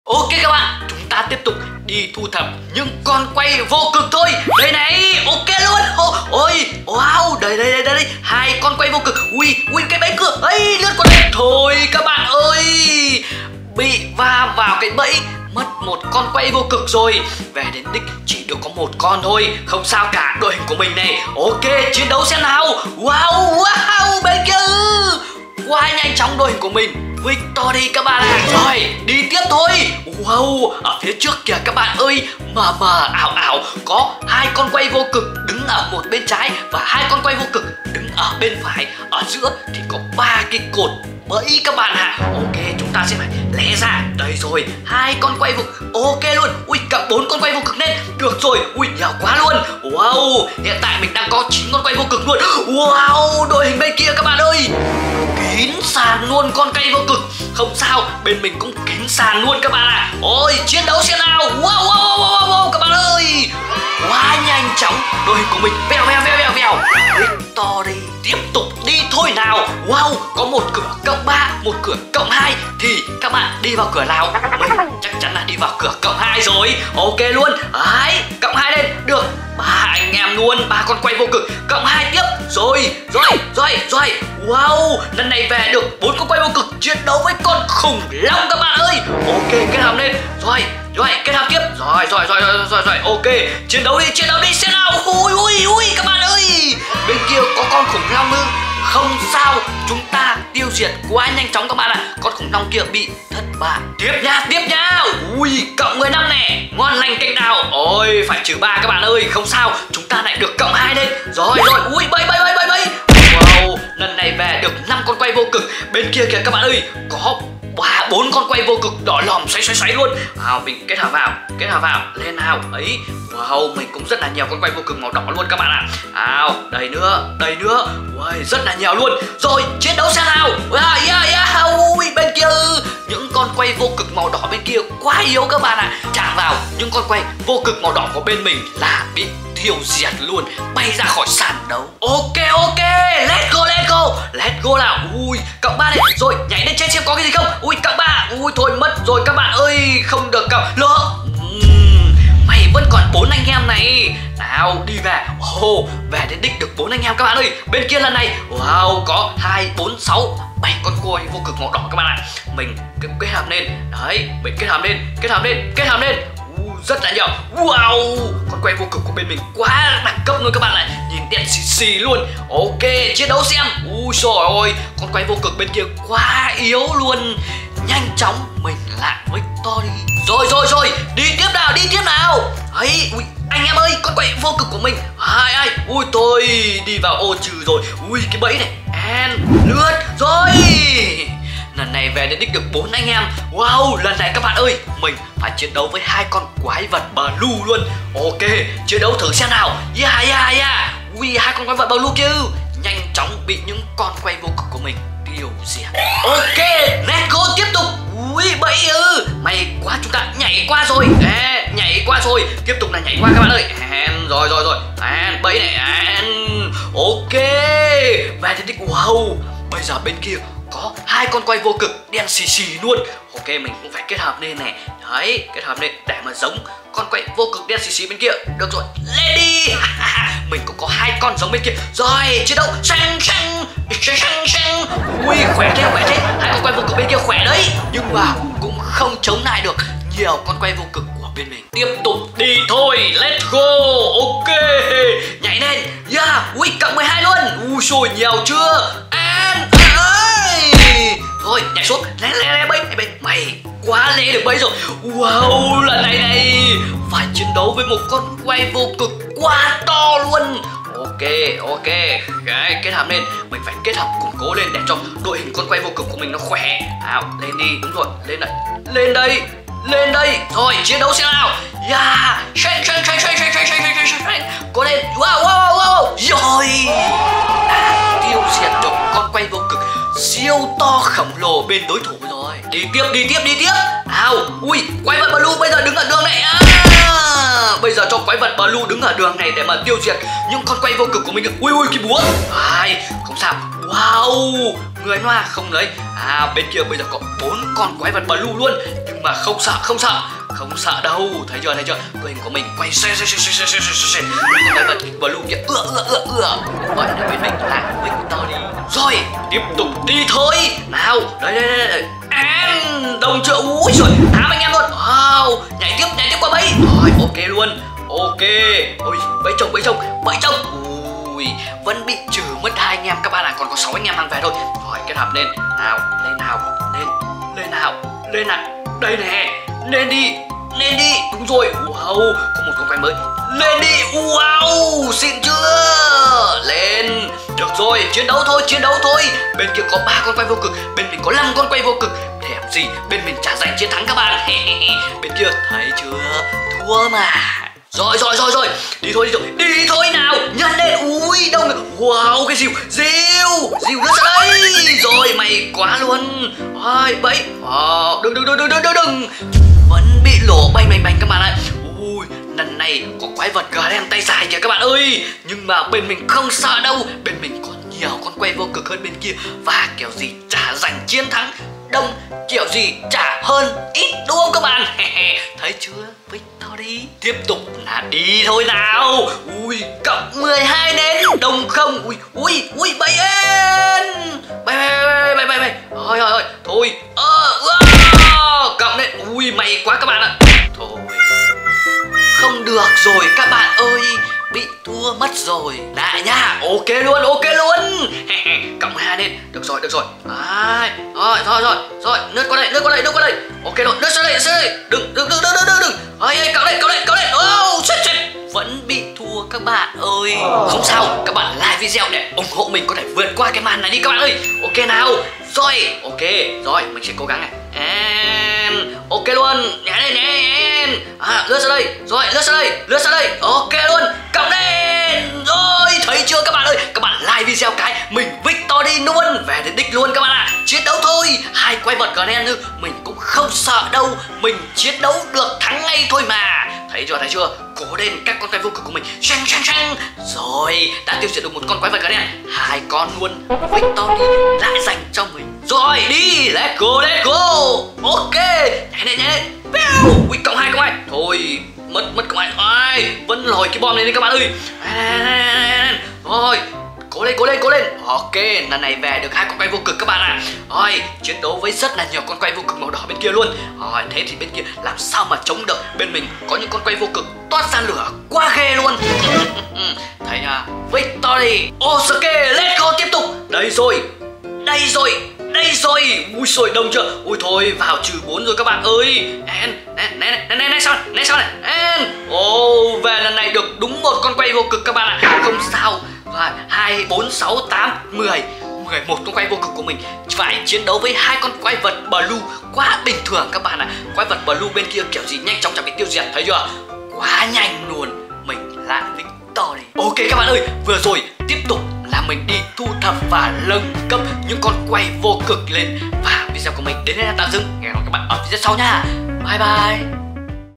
Thử Thách Quân Híp Nâng Cấp Con Quay Vô Cực Lưỡi Cưa Siêu Khủng _ Ai Dám Đấu ???
➜ 🚩 ẤN NÚT ĐĂNG KÍ
!! ĐỪNG QUÊN CLICK VÀO CÁI CHUÔNG 🔔 ĐỂ NHẬN NHỮNG THÔNG BÁO MỚI NHẤT TỪ KÊNH NHÉ !